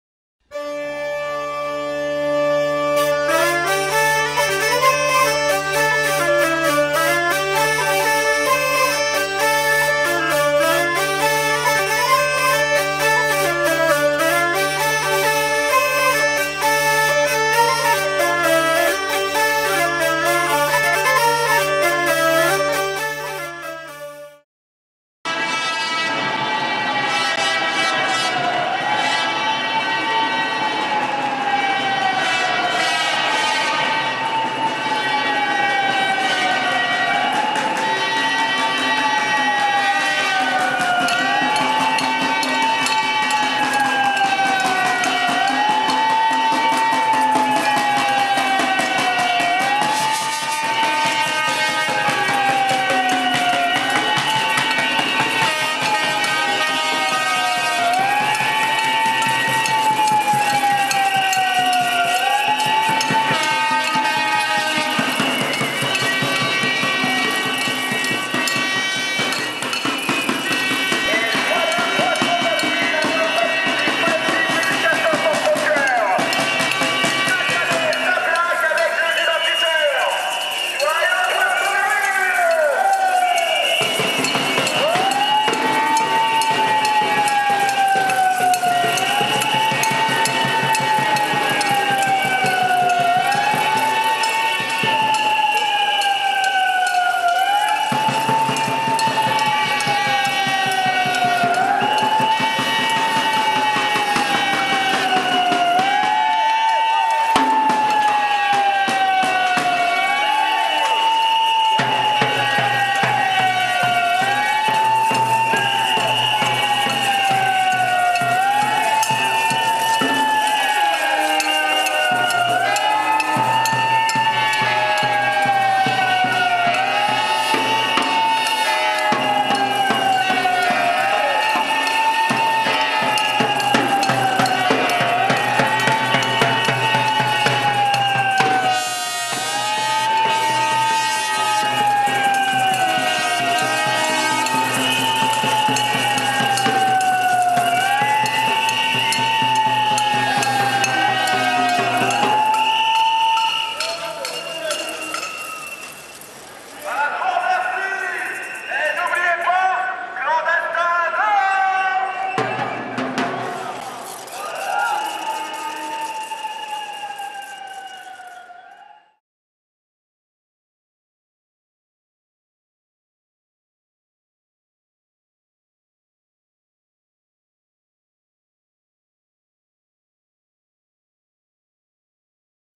Cercle du bruit VS Cercle du silence
Le réseau Education Sans frontières organisait un "cercle du silence à nice" pour protester contre les expulsions de clandestins. Mais un autre cercle, plus bruyant, fait son apparition.